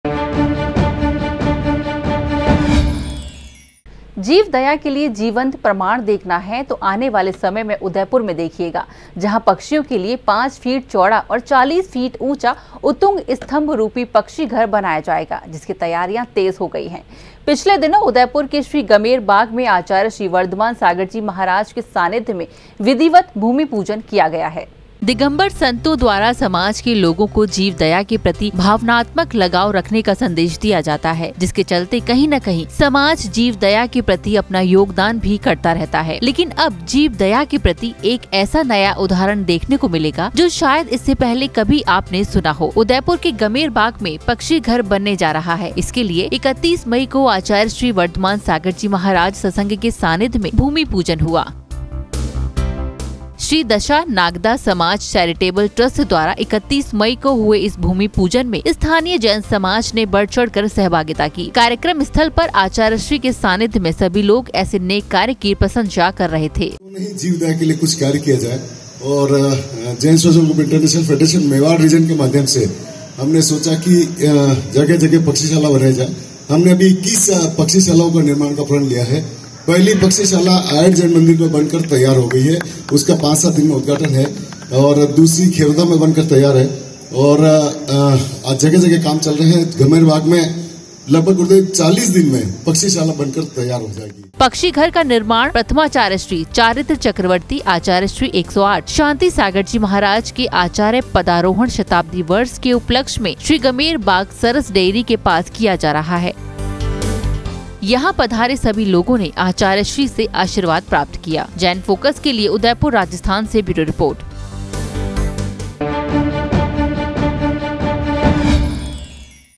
उदयपुर के गमेर बाग में बनेगा पक्षी घर | Udaipur News | JAIN FOCUS
उदयपुर के गमेर बाग में बनेगा पक्षी घर
आचार्यश्री वर्धमानसागर जी महाराज का मिला सानिध्य
आचार्यश्री के सानिध्य में 31 मई को हुआ भूमि पूजन
5 फीट चौड़ा और 40 फीट ऊंचा बनेगा पक्षी घर
जीव दया के क्षेत्र में उदयपुर समाज की अनोखी पहल